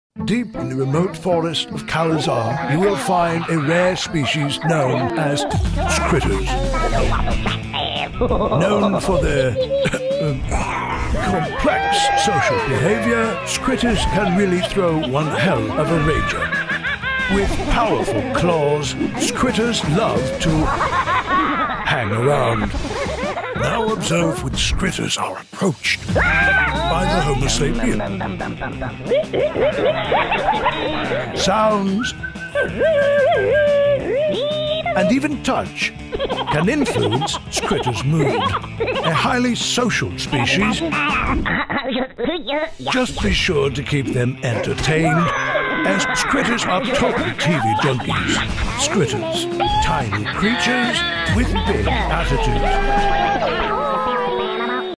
Scritterz (from Spinmaster!)
Scritterz are tiny jungle creatures that love to get a little wild and crazy!  Scritterz  react to sounds, tickling, and will become a little unpredictable when you put them together.  Clip 'em on your cloths, backpack, laptop, desk lamp...just about anything.  Use the Scritterz App to unlock more ways to play and control!!